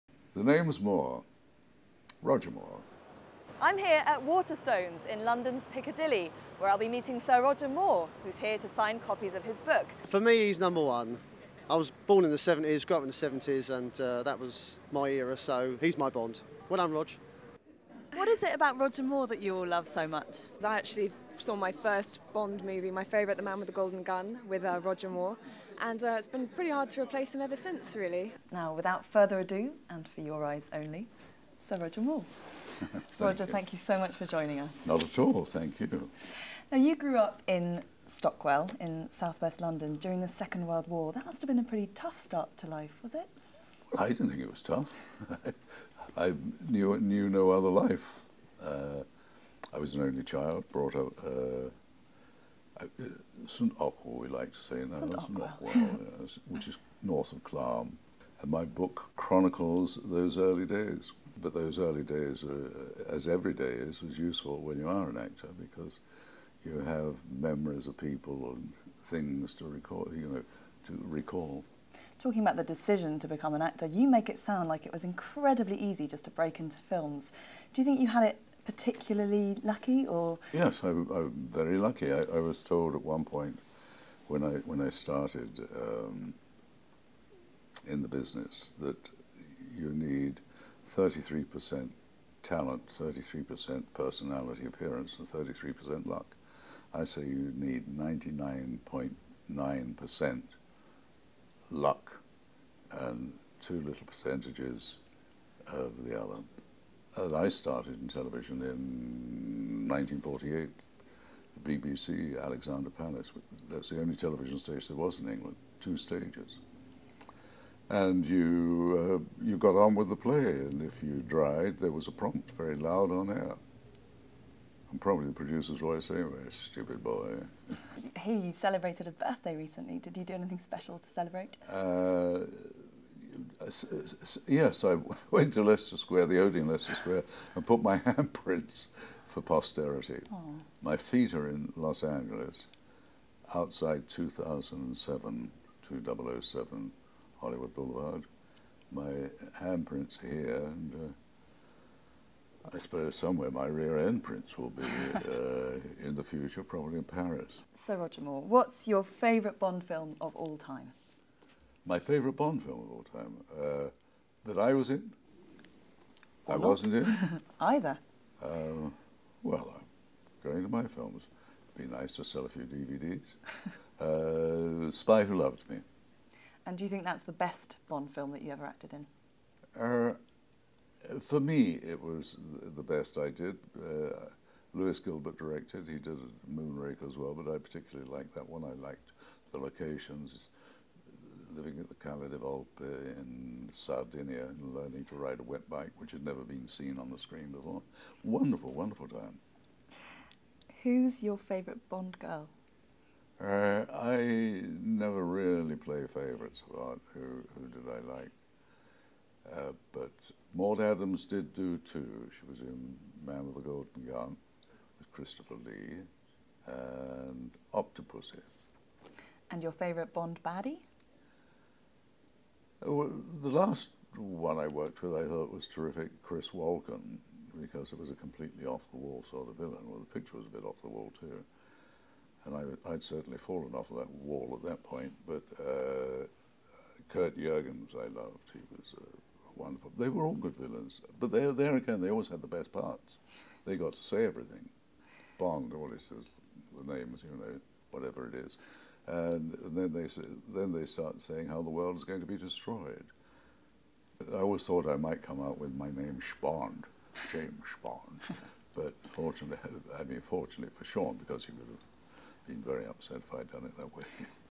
Sir Roger Moore - 'My Word is My Bond'
Sir Roger talks about his early life and career.